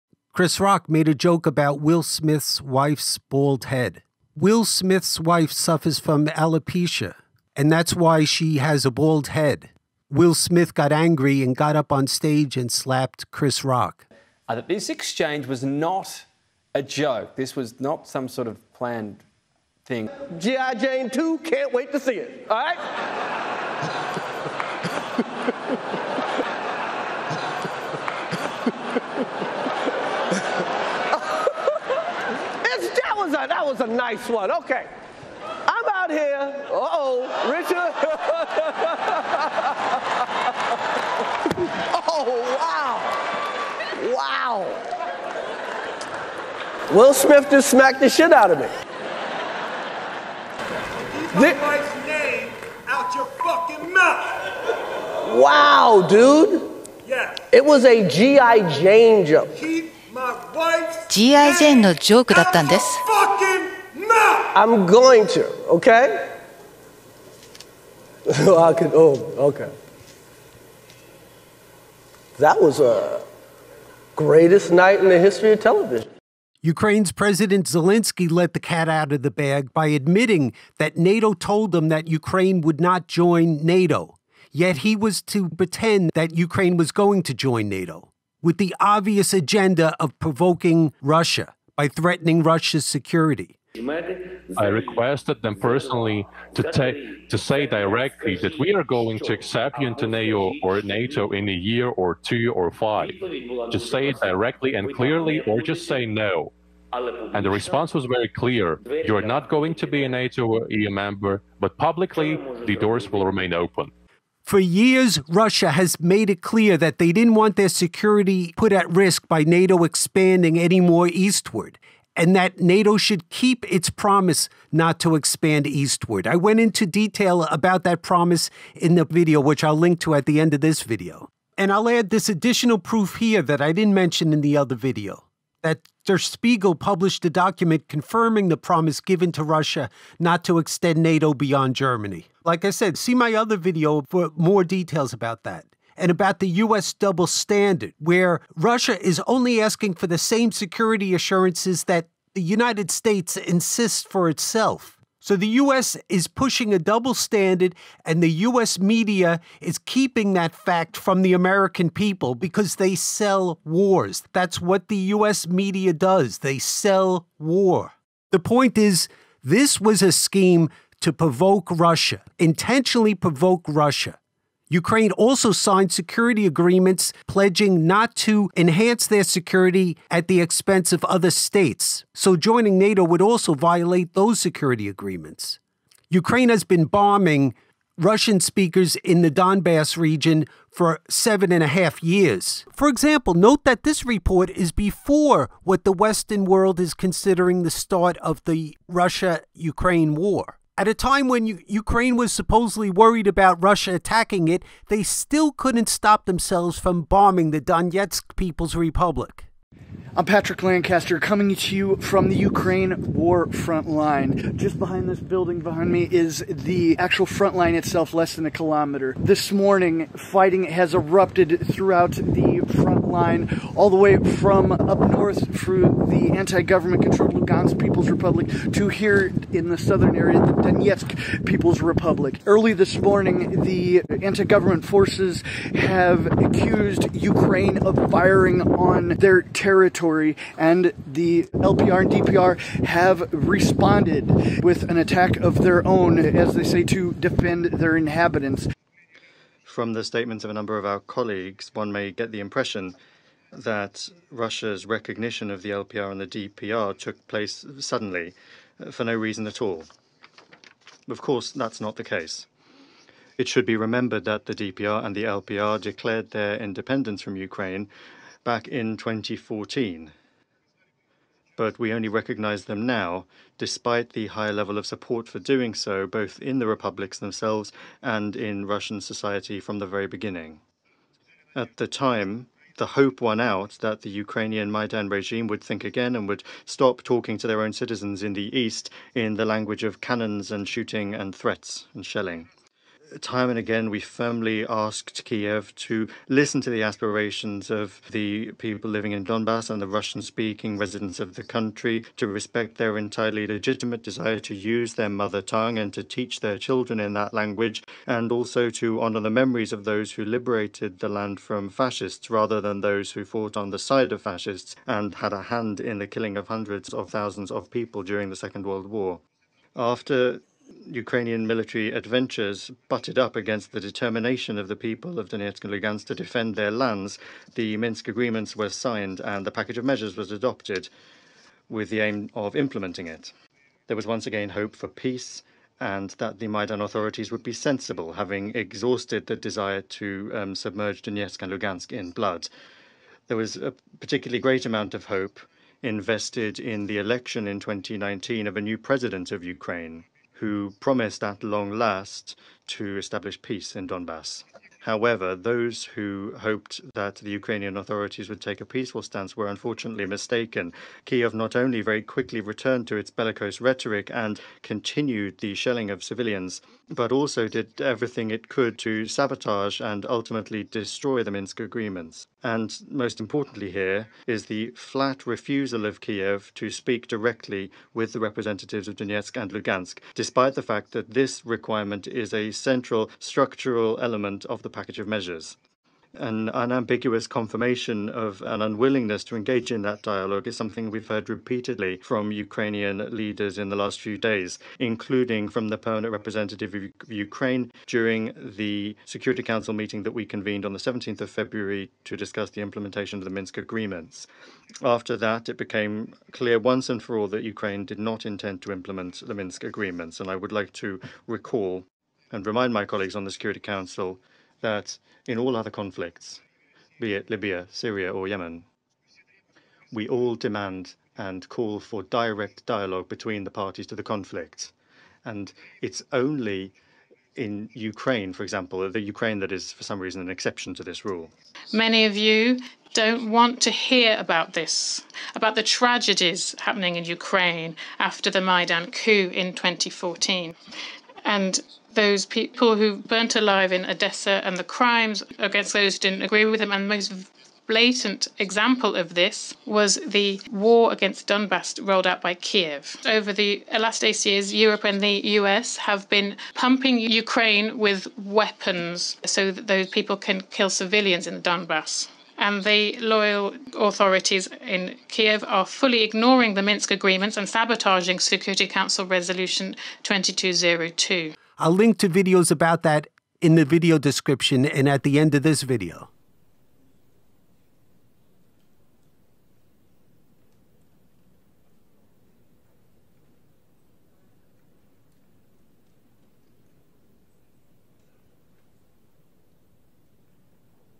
The Backstory of Why Will Smith Slapped Chris Rock & Bombshell Zelensky Admission!
A video analysis of news on Will Smith & Chris Rock and the bombshell revelation that Zelensky was duplicitous about NATO membership.

reporter Patrick Lancaster says "My report shows the horrible things that the Ukraine government did to some of the residents of the DPR yesterday 02/17/2022"

Chris Rock hasn't spoken to Will Smith and he didn't know Jada has alopecia